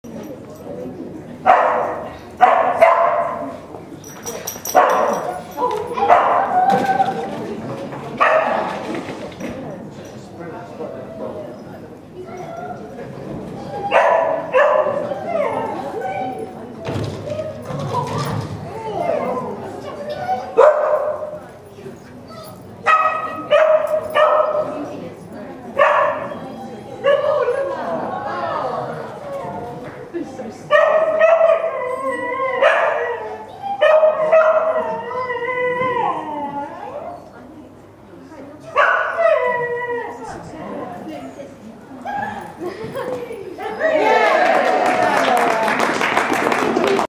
Nemo's Agility Course Night
Course night at the beginners Agility -  Billingshurst Dog Training Club on 17th March.  Shame about the first pole!  Nemo came 1st to win his third agility rosette!